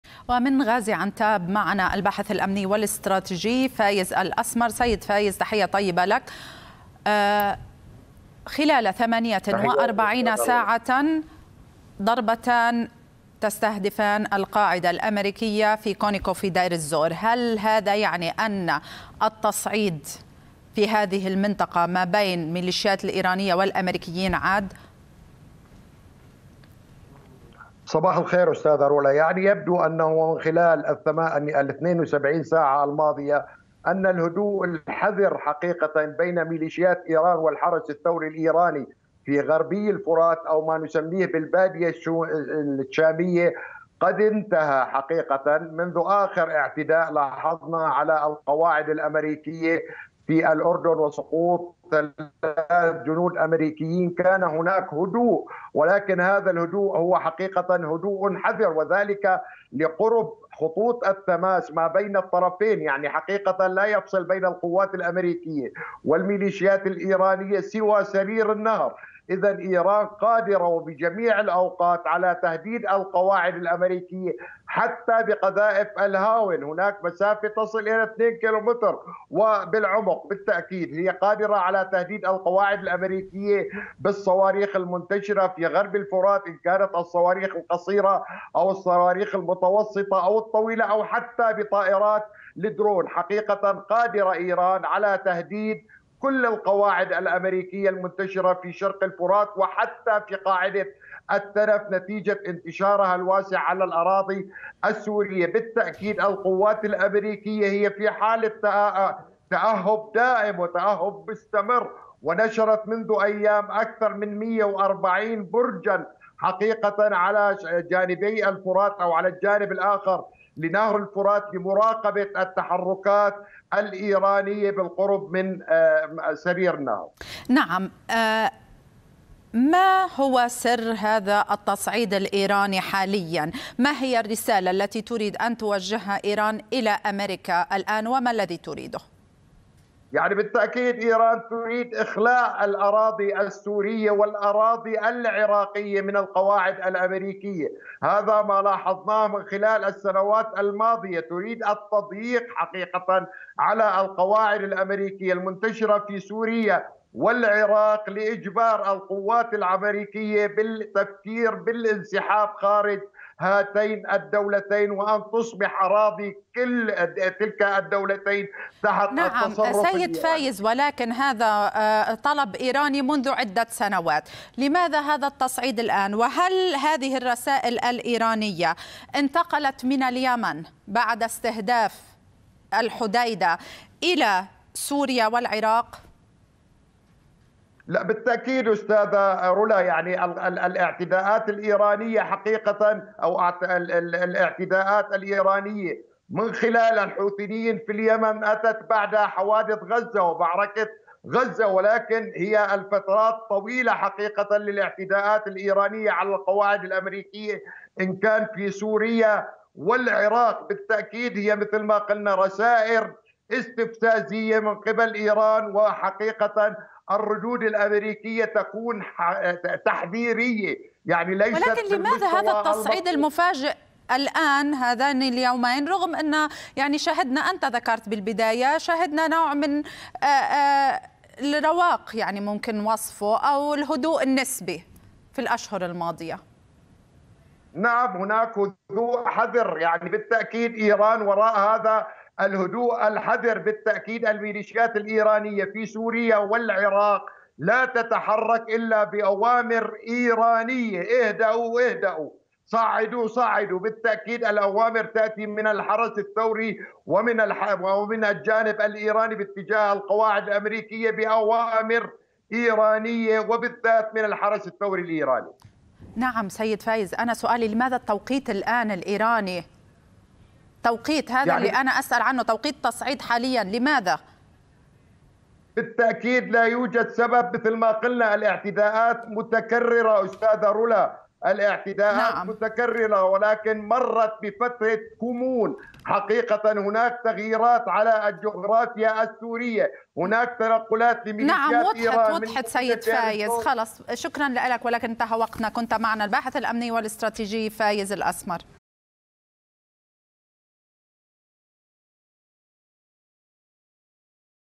باحث سياسي: إيران قادرة على تهديد القواعد الأميركية في شرق الفرات
الباحث في الشأن الأمني والسياسي فايز الأسمر: إيران قادرة على تهديد القواعد الأميركية في شرق الفرات وقاعدة التنف بشتى الأسلحة
لمعرفة أحدث المستجدات.. عبر البث المباشر من هنا
وبإمكانكم متابعة الحدث على منصات التواصل